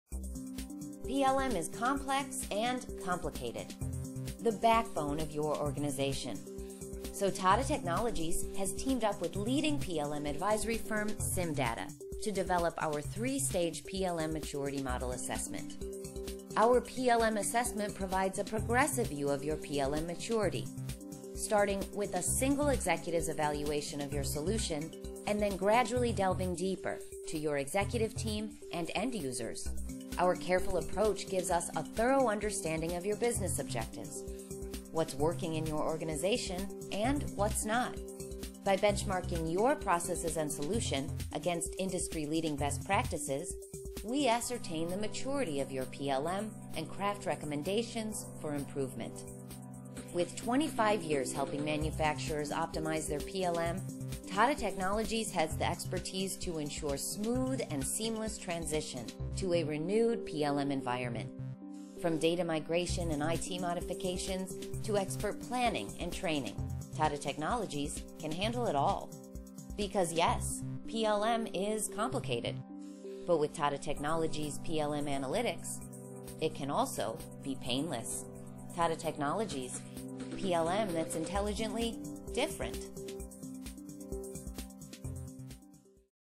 PLM Analytics Process
Our phased and deliberate methodology eliminates inconsistencies and provides assurance along the way that targeted benefits will be achieved.

We benchmark a manufacturer’s PLM against market-leading practices, which we have cataloged over the course of more than 250 PLM optimizations for global manufacturers. Considering the complexity of the typical PLM design, Tata Technologies’ structured and well-defined assessment provides an expeditious route to a realistic maturity level.